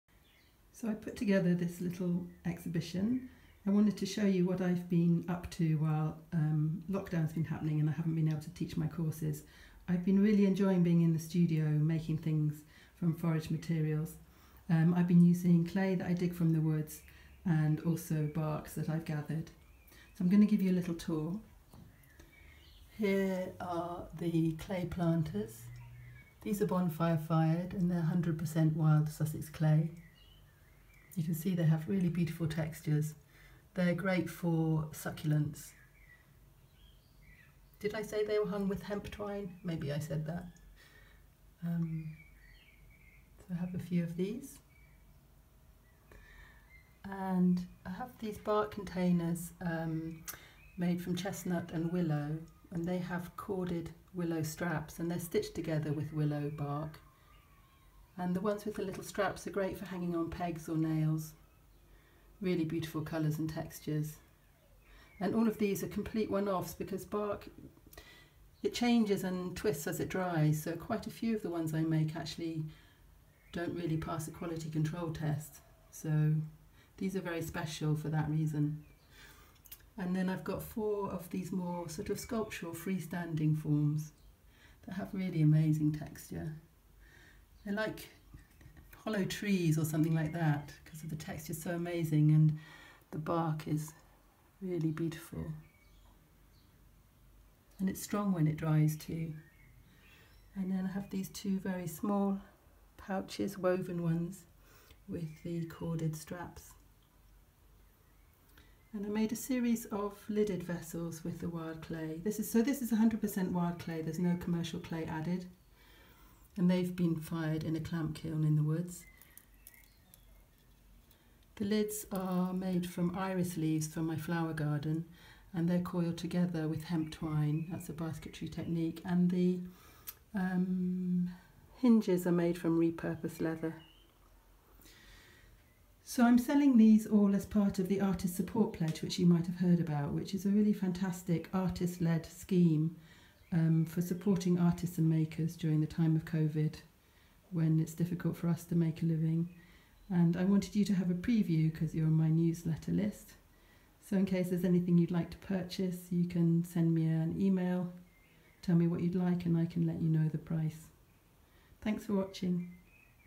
Preview of wild clay and bark vessels
Here's a private view I've created for you to see the pieces I'm offering for sale as part of the Artist Support Pledge*, as a preview to sales going live on Instagram this Sunday, 14 June 2020. They're all unique one-offs, and prices range from £35 - £150.
If you're interested in buying any of them, please let me know sharpish : )

All the pieces are made from foraged bark and wild clay, all sourced sustainably from the Sussex woods near where I live, and where I teach my courses. It's been wonderful to have dedicated studio time while I've been unable to teach my courses this spring.

Shipping/delivery, please note:
Bark pieces can be shipped to the UK, and shipping is included in the price. However, because the clay pieces are low-fired earthenware, I'll not be shipping these as I'd hate for them to break in transit. So if you would love one and are local, I'm happy to deliver free within a 10 mile radius of Brighton/Lewes, or you are welcome to collect from me in Lewes. Or I can hold pieces for collection at a later date. Please get in touch and we'll find a way to make it work : )

*Due to the COVID-19 pandemic, many artists around the world have found themselves without work, teaching, technical support, gallery work, exhibitions and sales have disappeared. In an attempt to help alleviate some of this stress @matthewburrowsstudio has instigated the artistsupportpledge
The concept is a simple one. Artists and makers post images of their work to sell for no more than £200 each (not including shipping.) Anyone can then buy the work. Every time the artist/maker reaches £1000 of sales they pledge to buy another artist's work for £200.